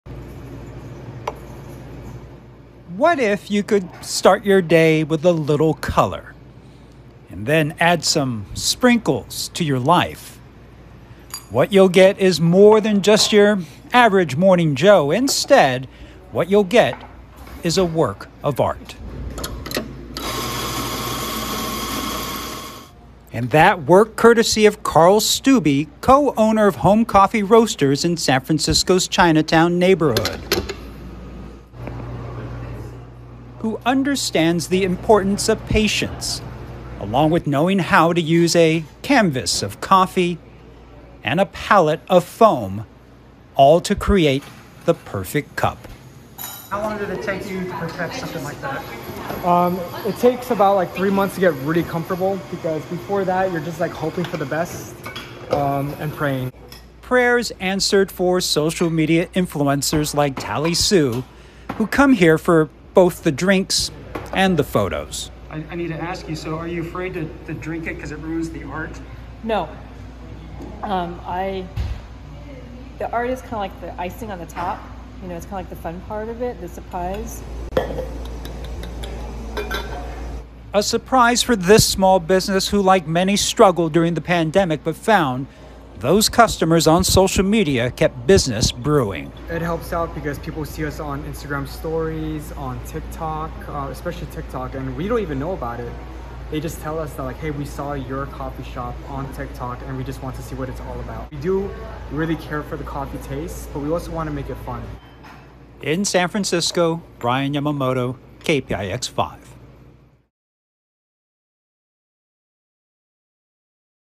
Coffee shop in SF's Chinatown lifts latte art to new level
The birthday cake latte at Home Coffee Roasters in San Francisco's Chinatown -- with rainbow art and sprinkles -- is almost too beautiful to sip ... almost. Ryan Yamamoto reports. (9-9-22)